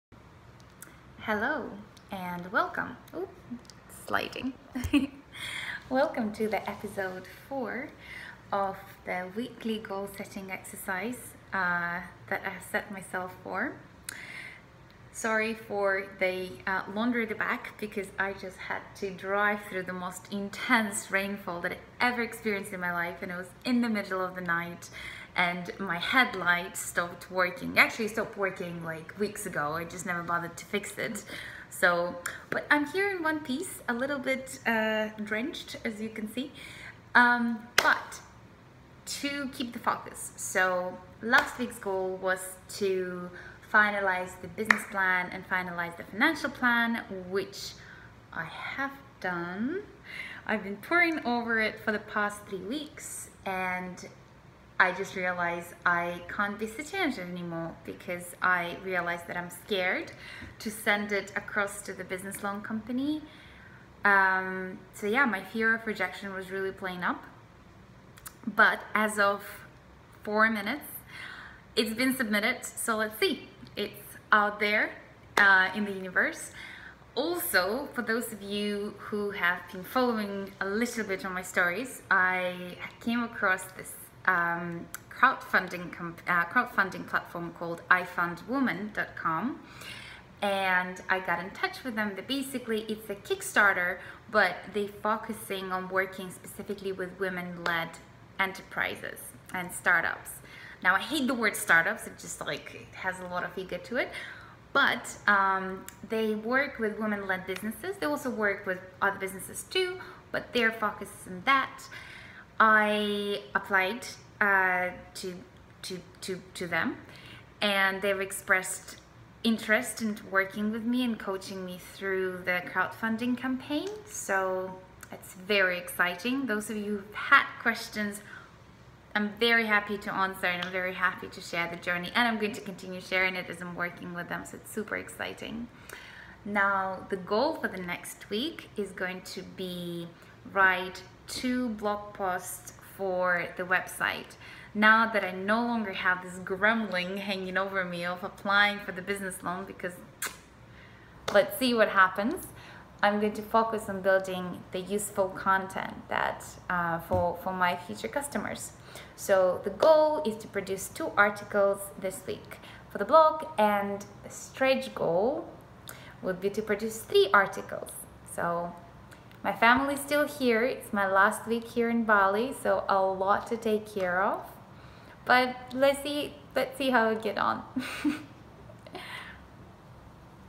How I launched The Hair Fuel: Episode 4
Episode 4: Business loans, crowdfunding platforms and survival with no headlights.

Last week goal was to finish up my business plan and financial model. In this episode I talk about how I went about achieving that and also other things I discovered along the way, including a surprising source of finance and tackling my own fears… Tune in to hear this incredibly wet (you’ll find out why) update.

Recap: this is a short series of 3 minute, unedited videos about how do I go about putting a hair growth mask on the market. Each week I set myself goals for the week and review its achievement next week. I give updates, share excitement, fears, worries, the “yay!” moments and thoughts. You can find out more about the product